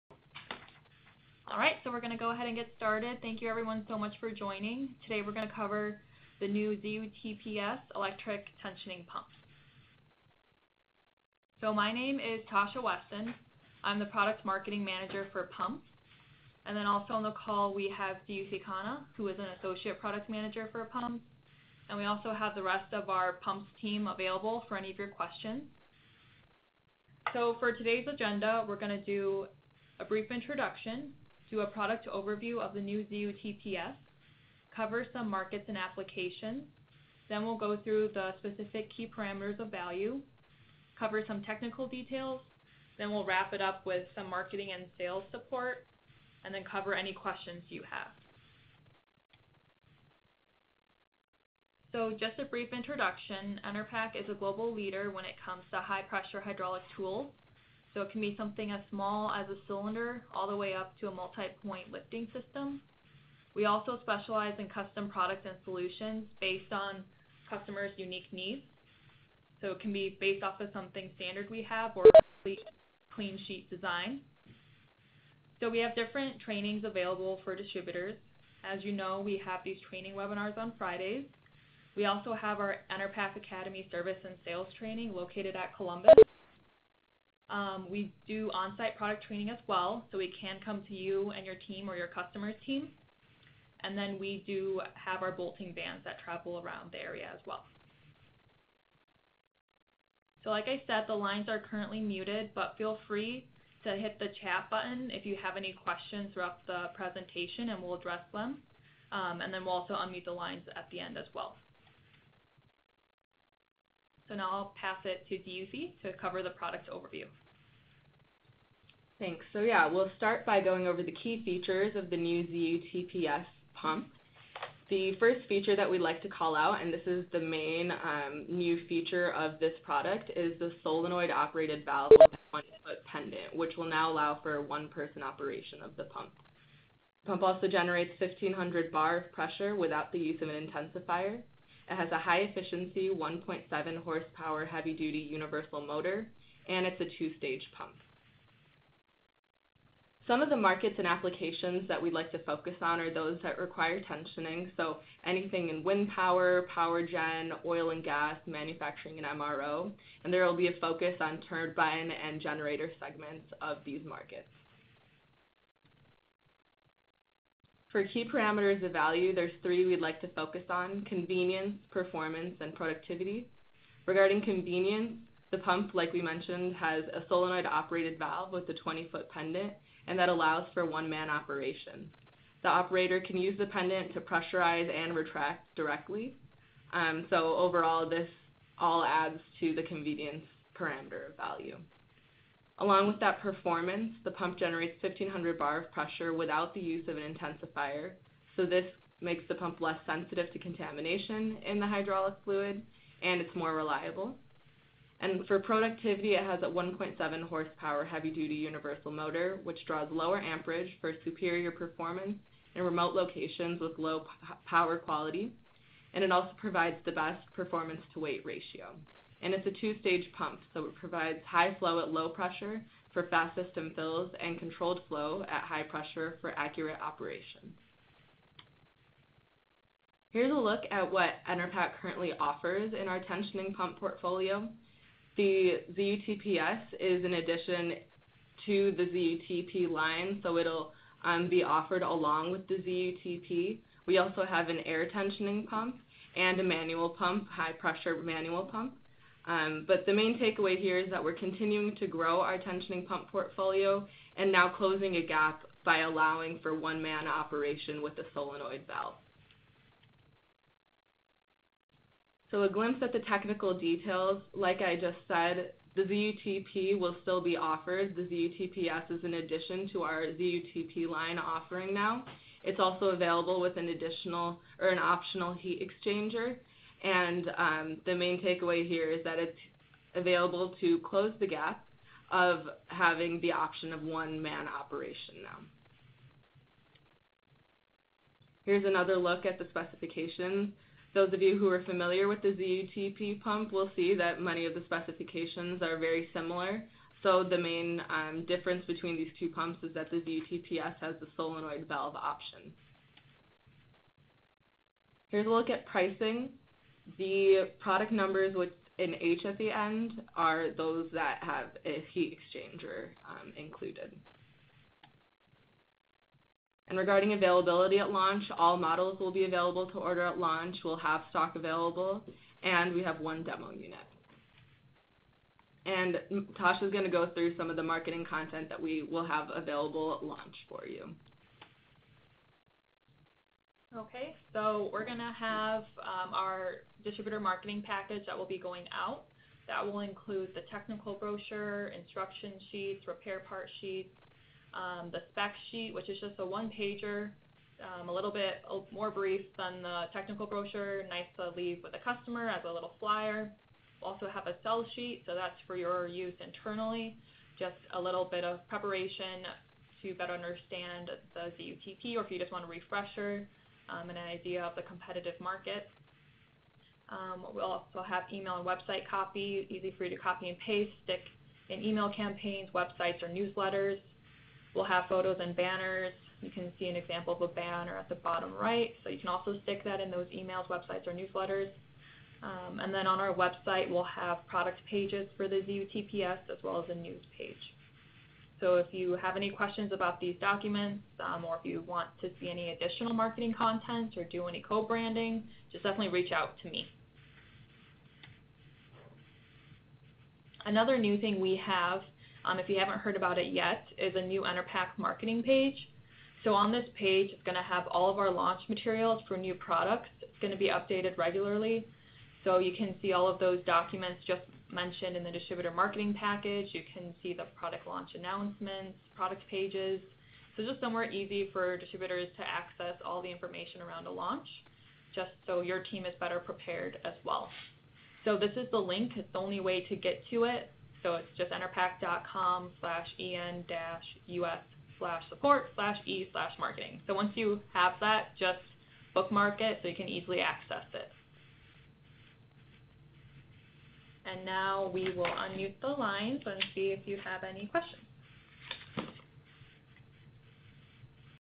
ZUTP-S Electric Tensioning Pumps - Training Webinar | Enerpac
The new ZUTP-S Series is an extension to our existing line of ZUTP electric tensioning pumps. The ZUTP-S Series features a pendant-operated solenoid valve which is ideal for multiple bolt tensioning applications and allows for single-person operation. The operator can pressurize and retract the tensioner directly from the pendant.